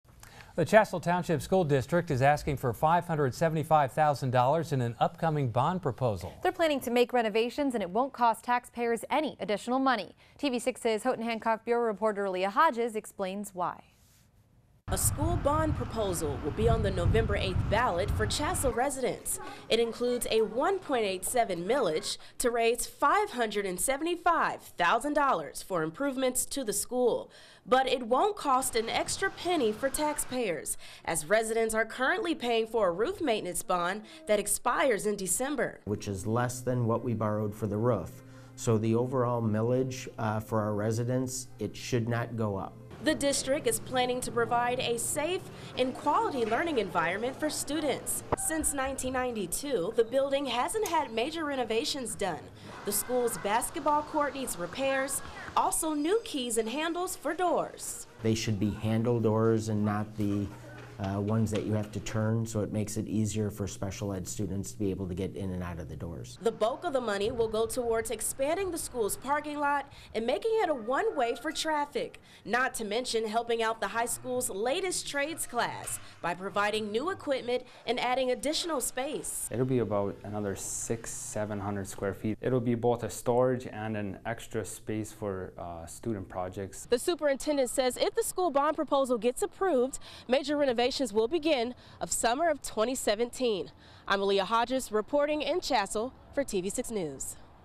Breaking down the Chassell Twp school bond proposal
Why it won't cost tax payers a dime.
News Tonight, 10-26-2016